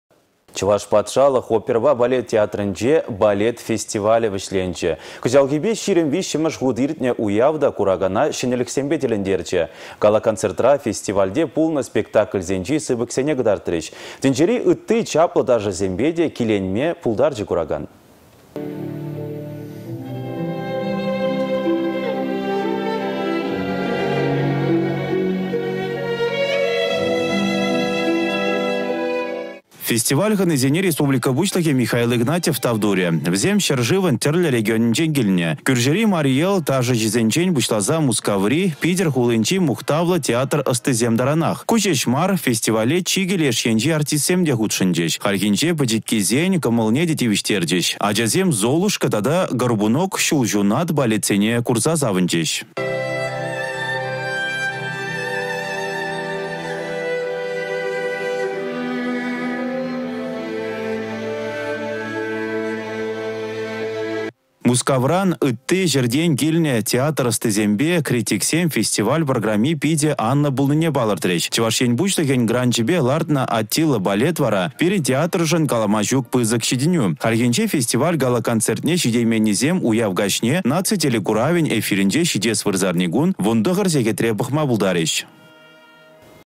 Балет фестивалĕ вĕçленчĕ
Чăваш патшалăх оперăпа балет театрĕнче балет фестивалĕ вĕçленчĕ. Кăçалхипе 23-мĕш хут иртнĕ уяв та куракана çĕнĕлĕхсемпе тĕлĕнтерчĕ. Гала-концертра фестивальте пулнă спектакльсенчи сыпăксене кăтартрĕç. Тĕнчери ытти чаплă ташăсемпе те киленме пултарчĕ куракан.